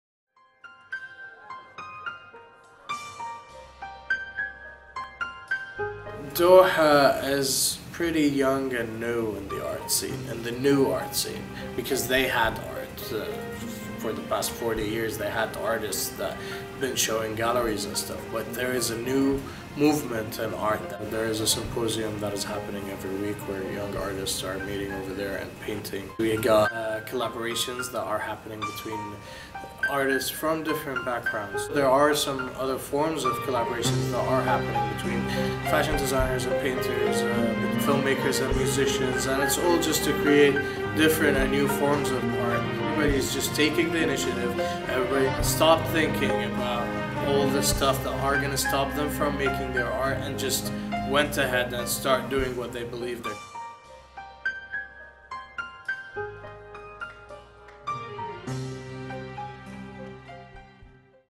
Artistic Dohati
Mohammed Alhamadi is a Senior Standards Engineer working in the Quality Assurance Department in Qatar Petroleum, and an art student in Virginia Commonwealth University in Qatar.

After graduating with a BS in Chemical Engineering from the University of Tulsa in Tulsa, Oklahoma, his love for art has led him to seek a BFA in Painting and Printmaking. He did some commission work as a painter and owned an art gallery/studio in Souq Waqif in Doha, Qatar.

He is also the CEO of Innovation Production a film production company based in Qatar. Mohammed is a Painter, a writer, a musician, a film maker, a business man, and an engineer.